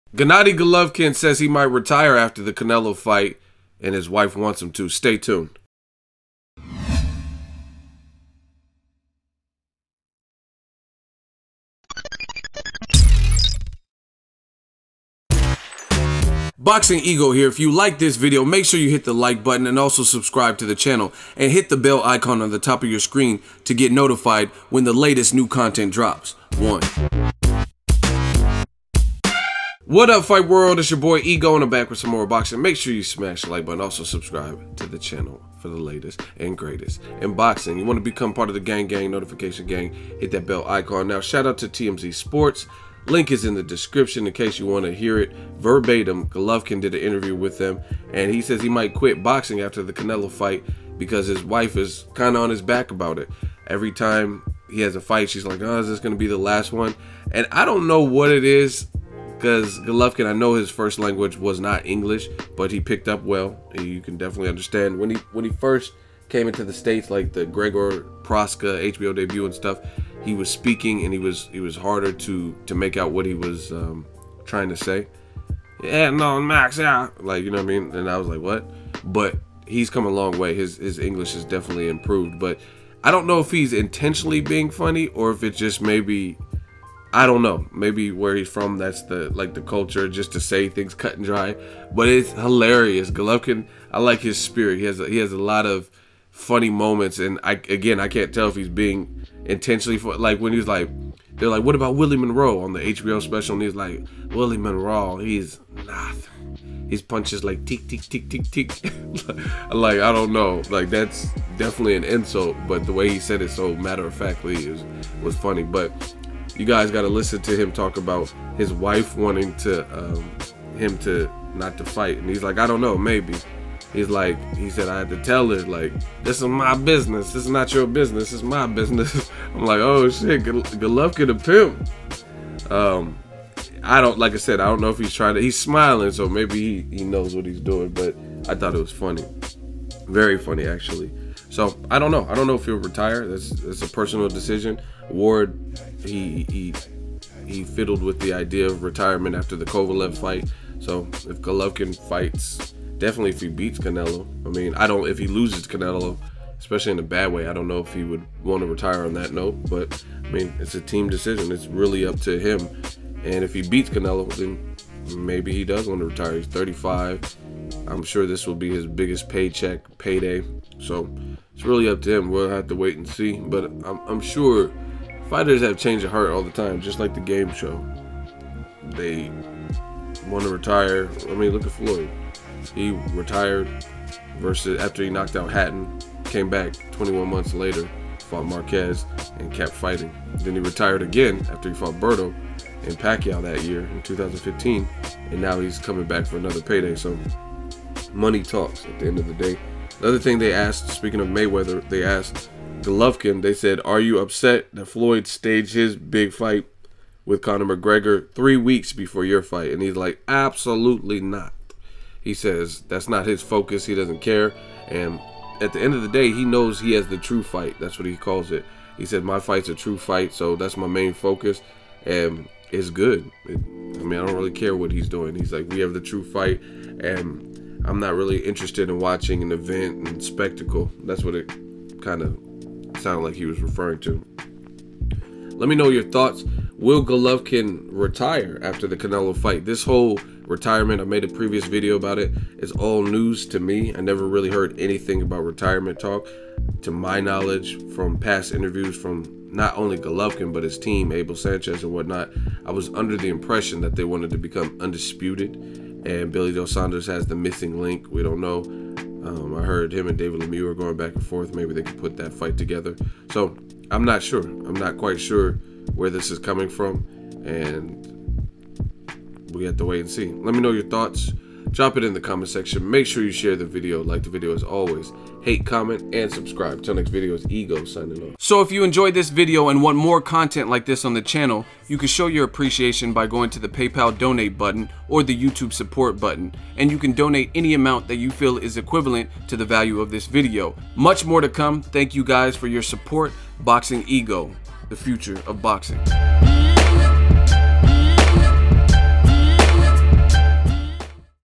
GGG MAY RETIRE AFTER CANELO FIGHT PER WIFE'S REQUEST
Gennady Golovkin tells TMZ sports he may retire from boxing per his wife's request. He mentions that his wife is constantly asking about when his last fight will be. EGO OF BOXINGEGO (FORMERLY EGOSWORLD1) reacts to Gennady GGG Golovkin's comments on maybe retiring after Canelo vs Golovkin fight and GGG's thoughts on Mayweather vs McGregor week's before his superfight.